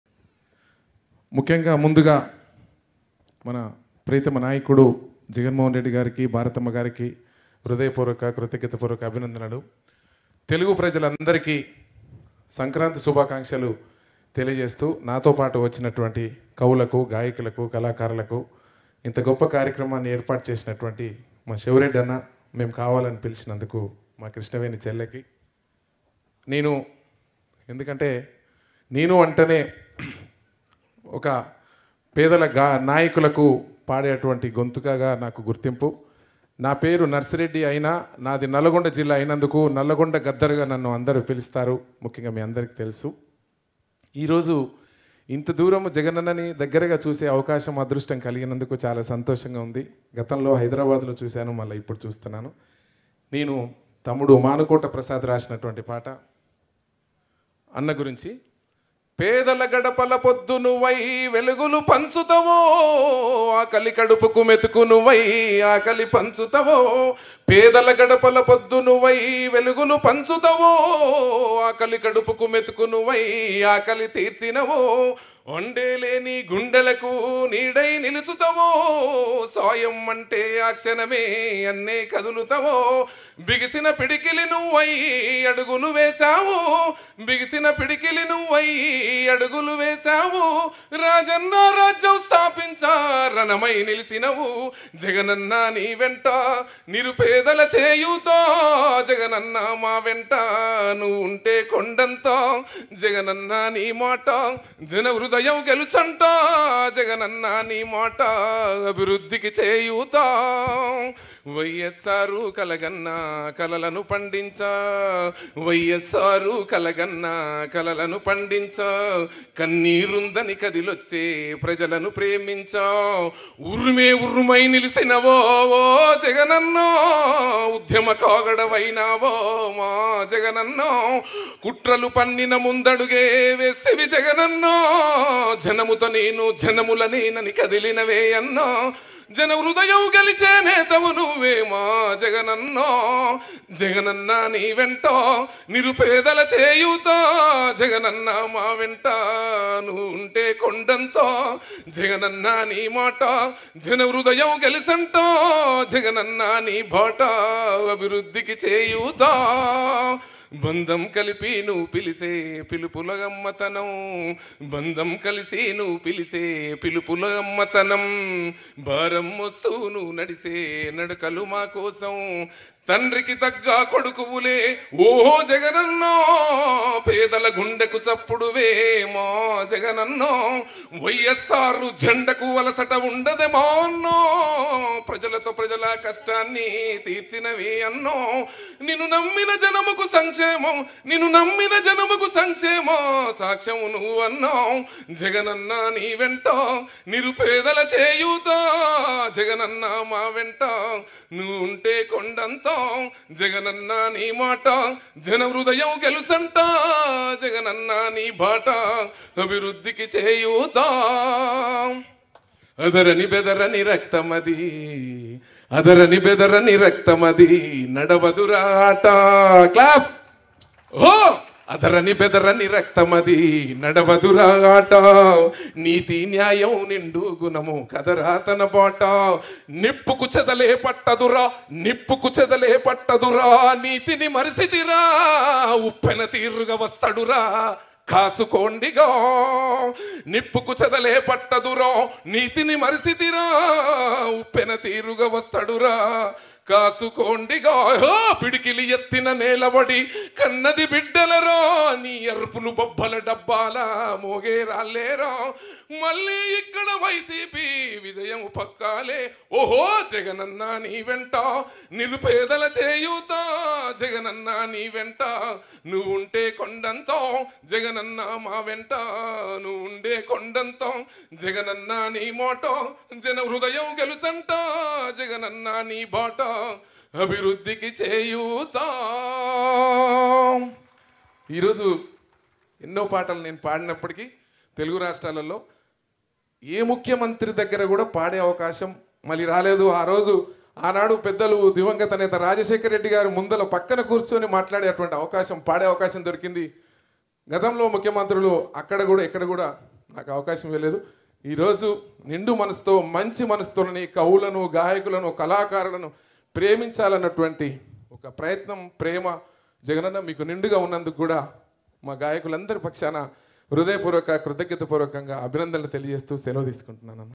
నాడు వైయస్ఆర్ సమక్షంలో.. నేడు సీఎం జగన్ సమక్షంలో పాట పాడే అవకాశం | Nalgonda Gaddar
నాడు దివంగత వైయస్ఆర్ సమక్షంలో.. నేడు సీఎం వైయస్ జగన్ సమక్షంలో పాట పాడే అవకాశం వచ్చింది

కవులు, కళాకారులు, గాయకులను ప్రోత్సహిస్తున్న సీఎం వైయస్ జగన్ గారికి అభినందనలు

-ప్రముఖ గాయకుడు నల్గొండ గద్దర్